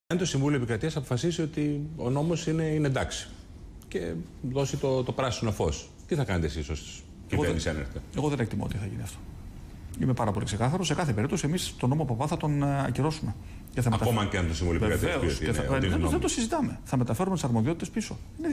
Μητσοτάκης: Θα καταργήσω τον νόμο ακόμα κι αν το ΣτΕ τον κρίνει συνταγματικό
Μητσοτάκης: Θα καταργήσω τον νόμο ακόμα κι αν το ΣτΕ κρίνει πως ο νόμος είναι συνταγματικός